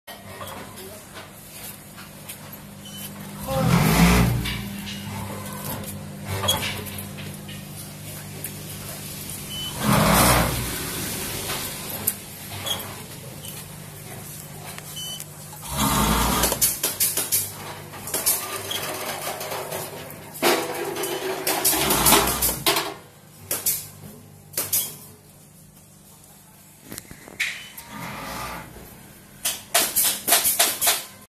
Powder quantitative packaging machine from ivory
1. The computer weigher ensure the stable and accurate packaging weight.
2. Medium-speed and high-speed machine can be selected, meeting different production requirements.
3. The packaging machine can be equipped with a variety of functional options, such as Alphascope, punching etc.
4. The computer weigher can be installed with hopper as required and suitable for different material.